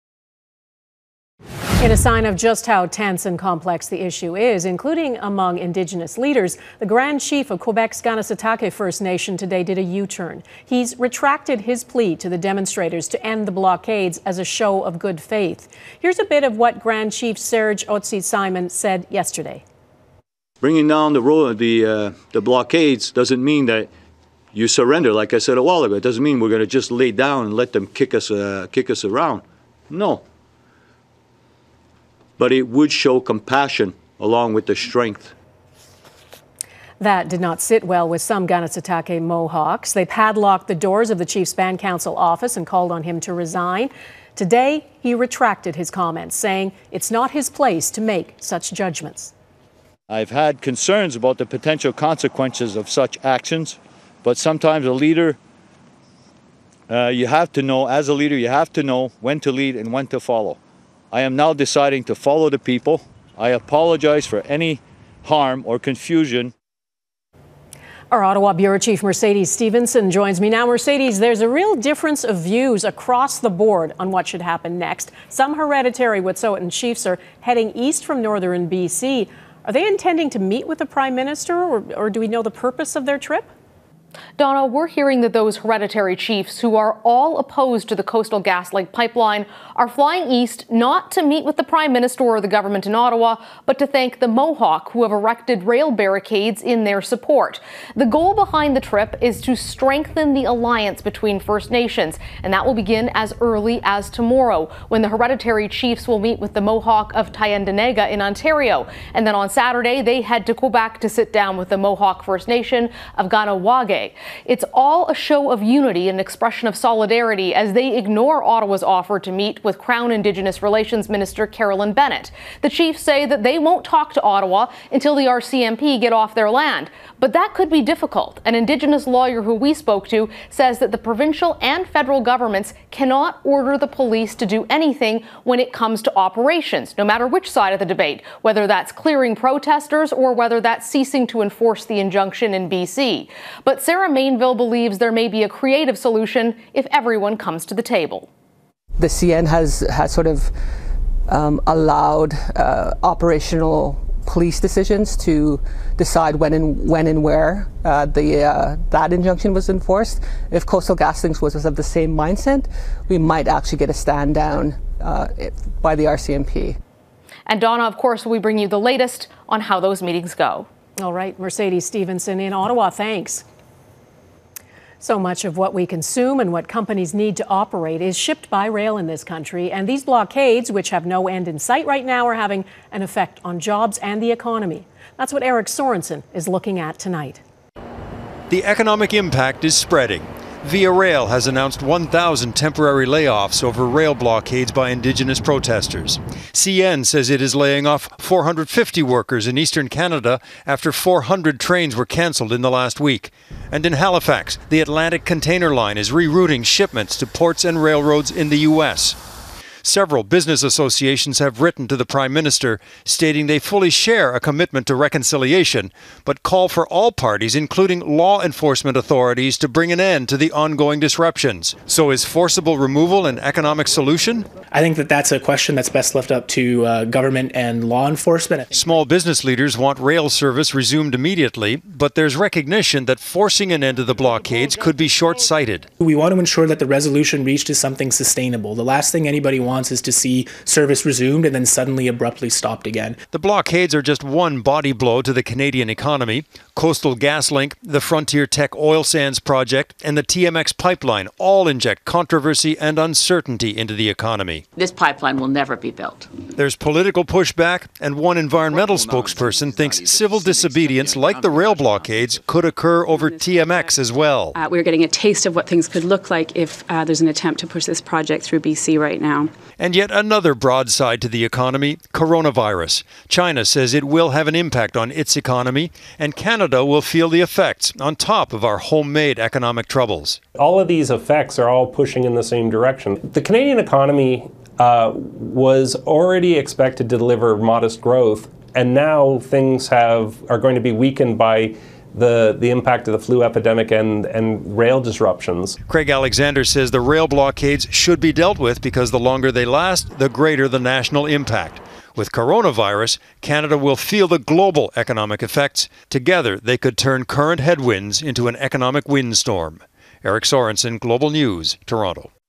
Canadian rail blockades: What kinds of challenges are being faced by feds, economy?
As pressure mounts on Ottawa to end the rail blockade crisis in Canada, Mercedes Stephenson looks at how the government can't order police to take action, and explains how one lawyer believes there may be a creative solution if everyone comes to the table.

The ongoing rail blockades over B.C.'s Coastal GasLink pipeline is affecting jobs and the economy. As Eric Sorensen explains, the domestic troubles are expected to worsen as the world braces for the economic impact of the COVID-19 outbreak.